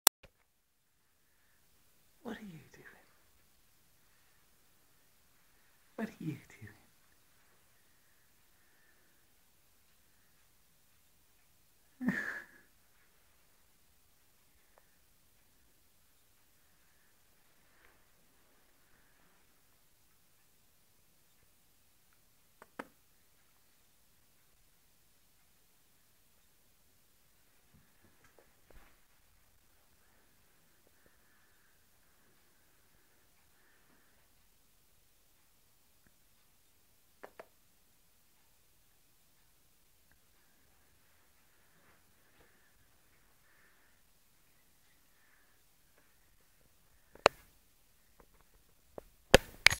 Cat in a christmas tree
Silly Mr Ferris climbing the christmas tree and feuding with decorations